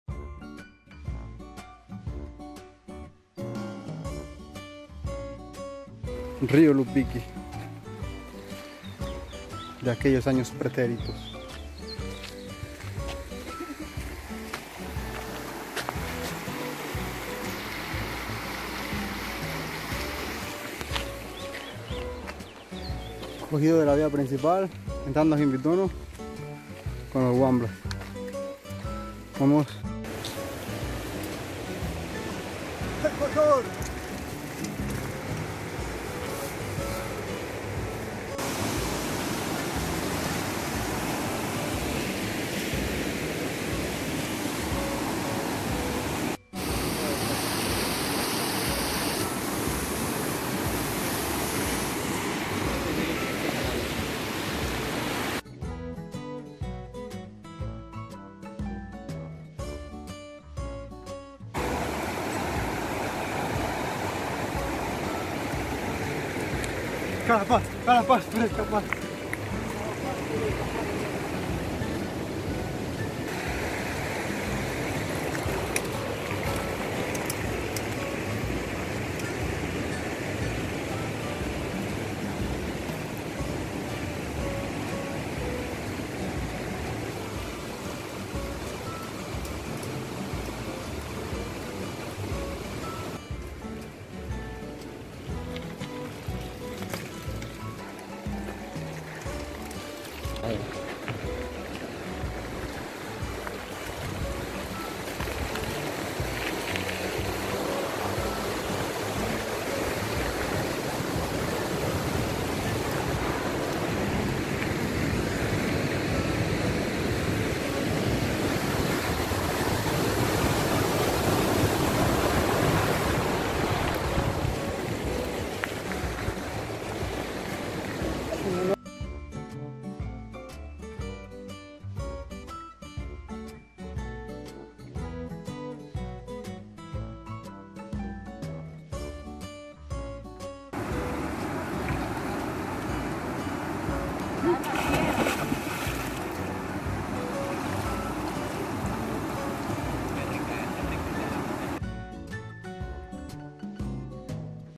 Macas - Jimbitono Ciclo Paseo 2021
Paseo en Bicicleta a las pozas de Jimbitono en Macas-Ecuador.